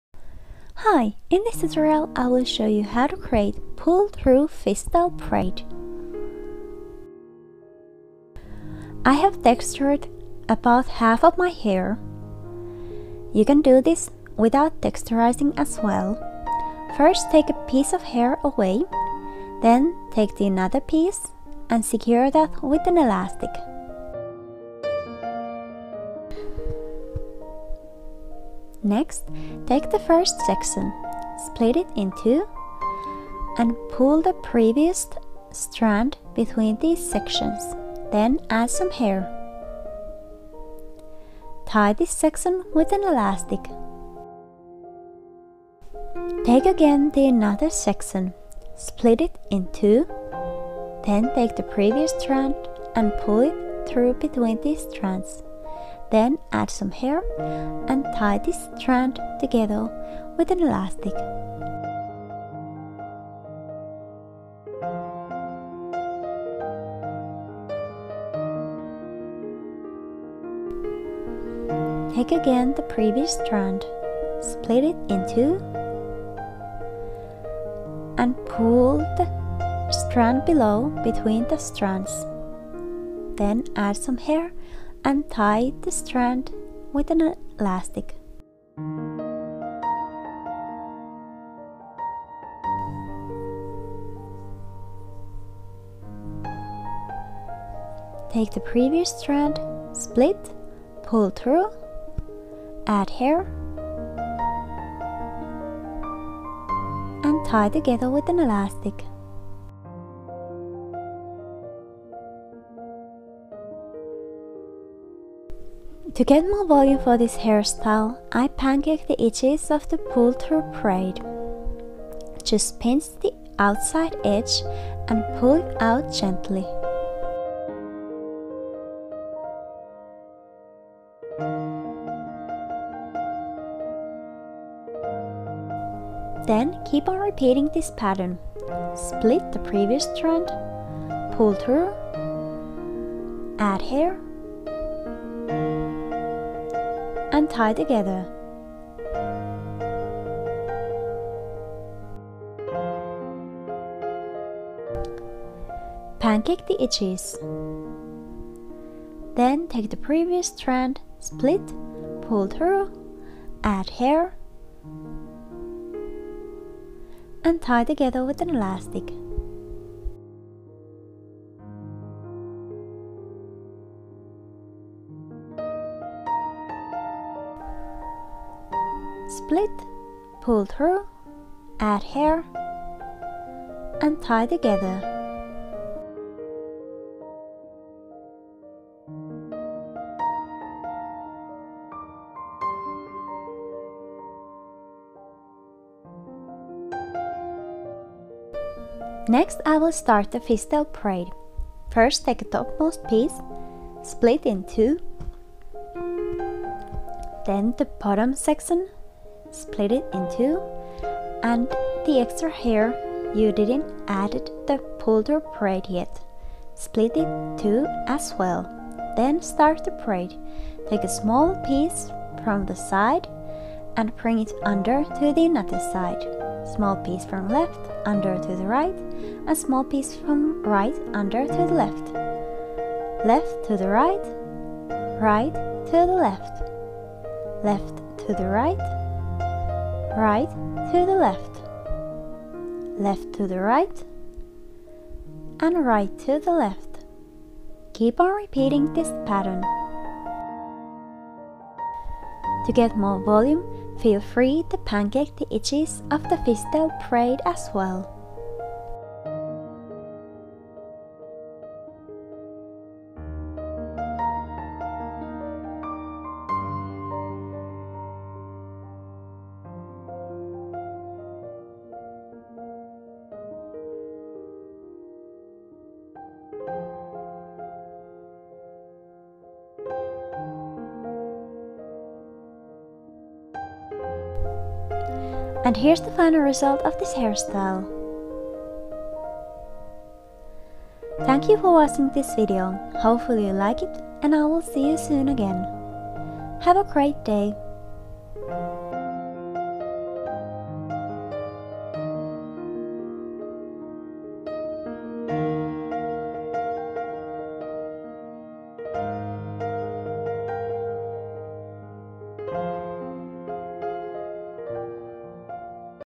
Pull through Fishtail Braid Combo | Easy Hairstyles
New easy hairstyle! Using 2 simple techniques you can create this braid combo easily to yourself or for your friend! You need some elastics and 2 strand braids! Pull-through braid is perfect for first braid cause you don't need to braid actually at all! Fishtail braid is also very simple and easy so great braid to start braiding! Braided hairstyles last time and use, you don't need to stress how your hair looks like!